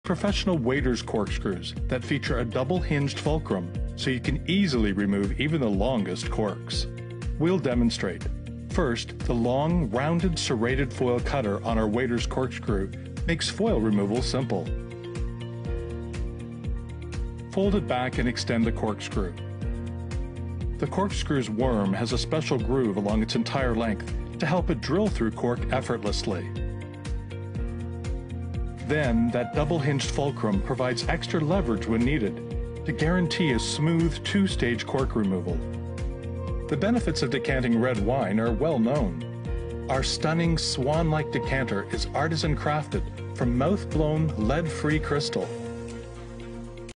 Corkscrew Bottle Opener
Waiter's corkscrew with double hinged corkscrew.
Use it to open bottles professionally with incredible ease.
Features ergonomic grip, stainless steel serrated foil cutter blade, spiral corkscrew, metal handle, and bottle opener.